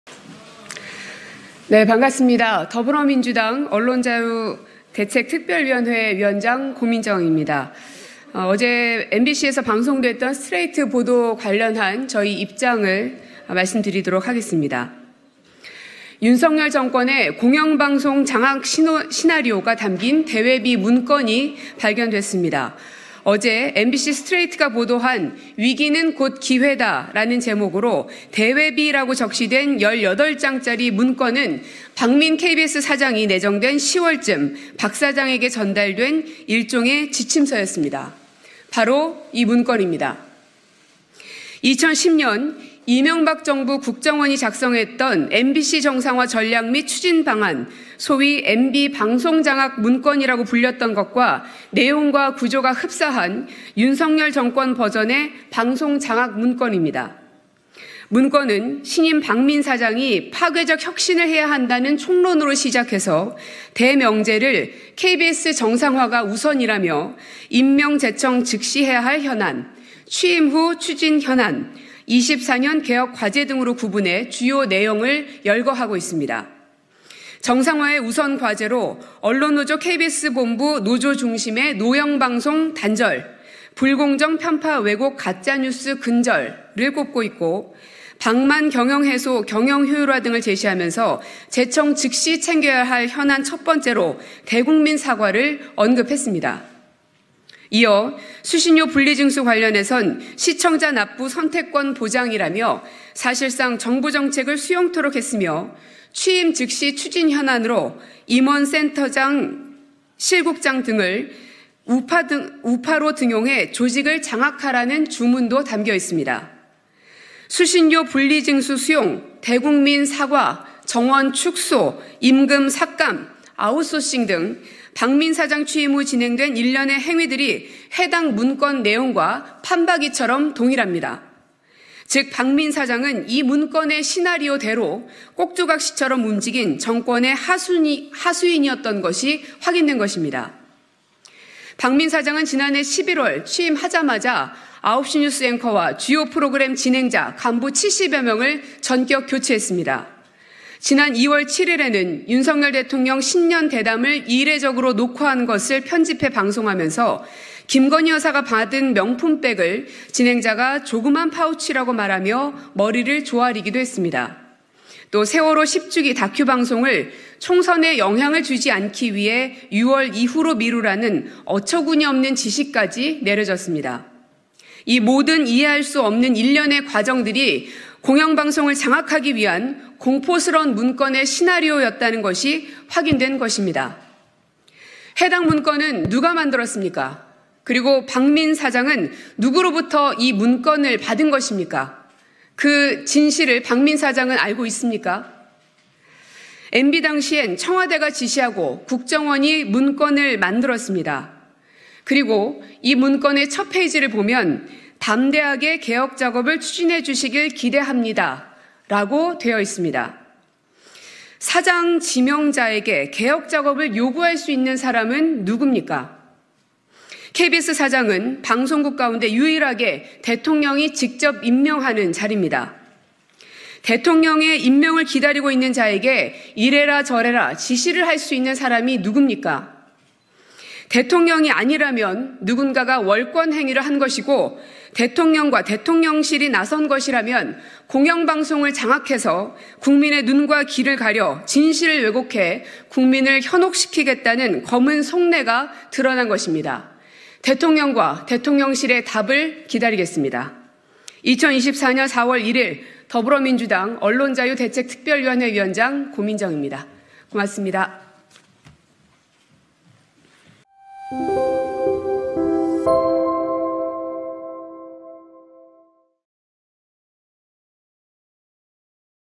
윤정권 ‘KBS 장악문건’ 실체에 분노 폭발한 KBS 출신 고민정 “박민 지시할 자는 대통령뿐...윤석열 입장 밝혀라!
2024년 4월 1일 국회 소통관에서
더불어민주당 고민정 의원(언론자유대책특위)이 윤석열 대통령 공영방송 장악 문건 실체 공개 요구 및 KBS 박민 사장 사퇴 촉구 브리핑을 진행하였다.